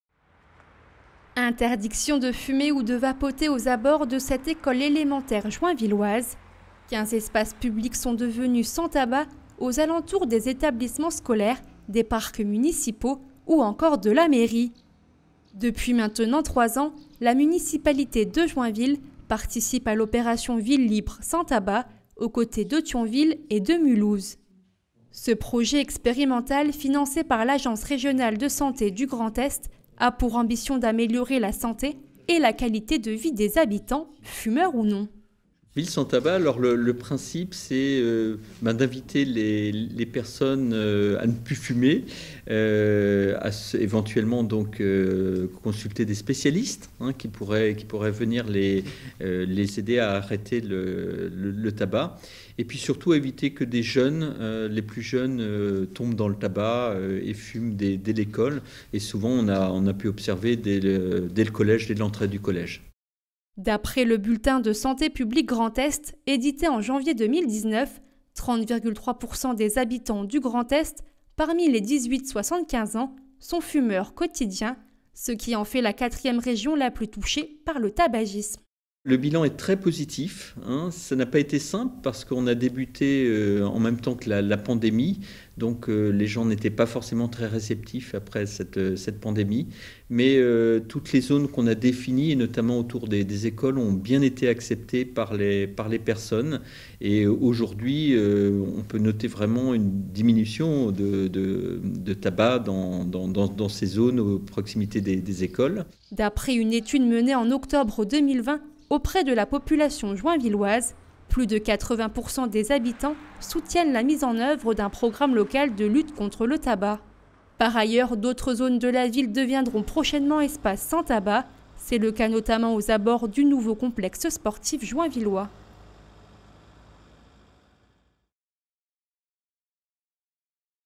Joinville : “Ville Sans Tabac”
Dans le cadre d’un appel à projets lancé, en 2019, par l’Agence Régionale de Santé du Grand Est, Joinville a été choisie comme ville-pilote pour expérimenter l’opération « Ville Libre Sans Tabac ». Parmi les mesures mises en place, quinze espaces publics sont devenus sans tabac.